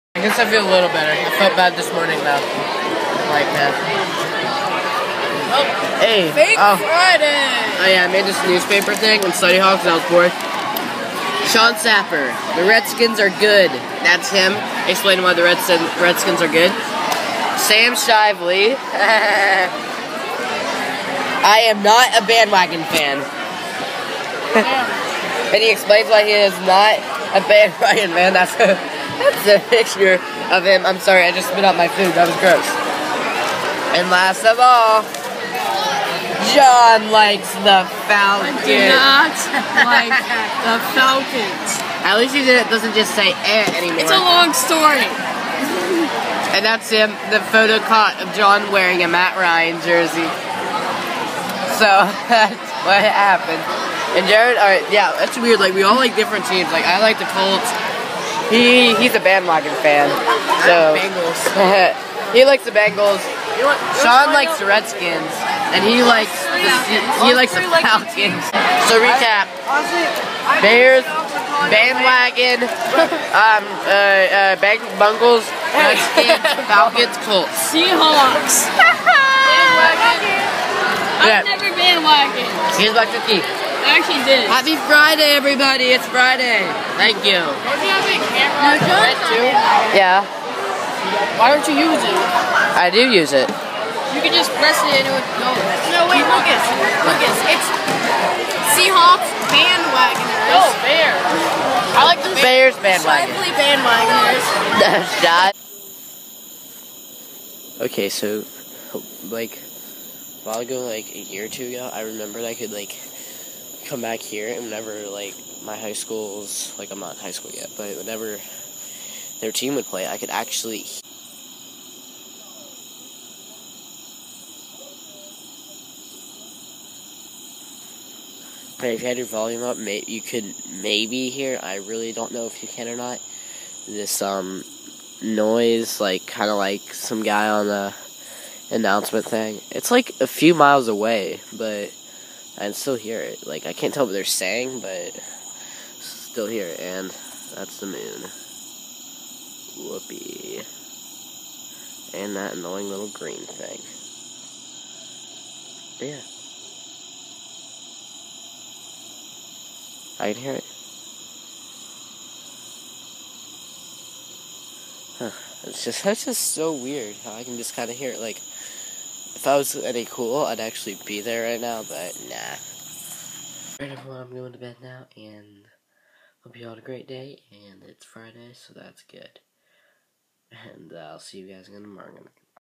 I Can Hear It (9/5/14 - Day 169)
We talk about our favorite football teams and I try to overhear my High School teams football game.